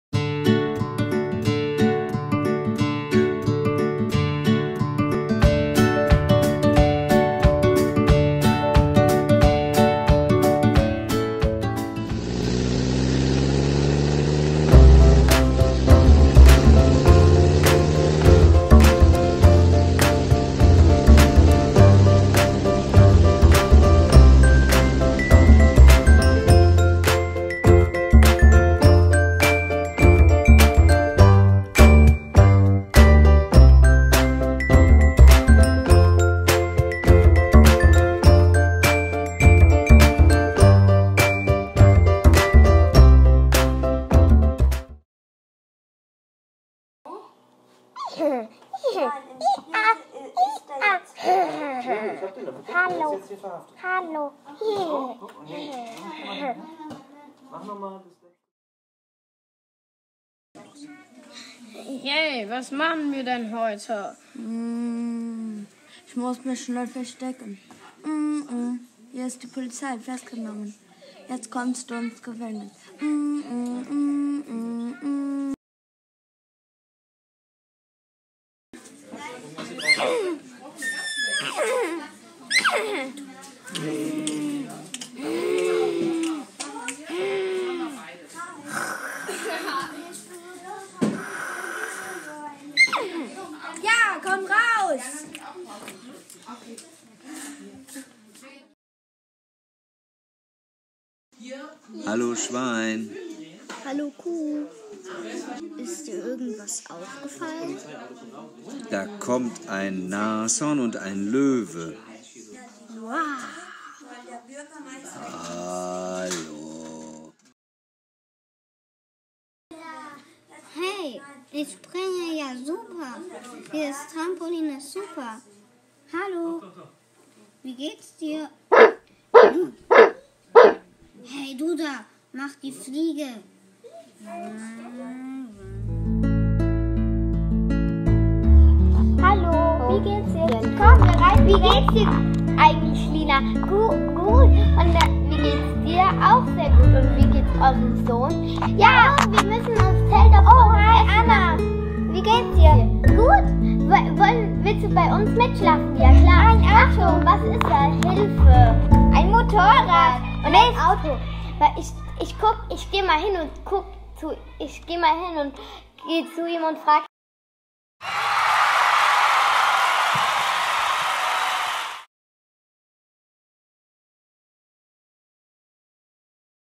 Trickfilmchen der Brüder-Grimm-Schule in Friedberg
Die Trickfilmchen sind entstanden auf einem Eltern-Kind-Nachmittag im Rahmen des hessischen Projekts Internet-ABC-Schulen 2018. Das ist ein Projekt der LPR Hessen in Kooperation mit dem Hessischen Kultursministerium und der Medieninitiative Hessen Schule@Zukunft. Die Eltern Kind Nachmittage werden vom Verein Blickwechsel e.V. durchgeführt. Zeit für die Umsetzung: 45 Minuten! Toll gemacht, Kids und Eltern! Mehr Infos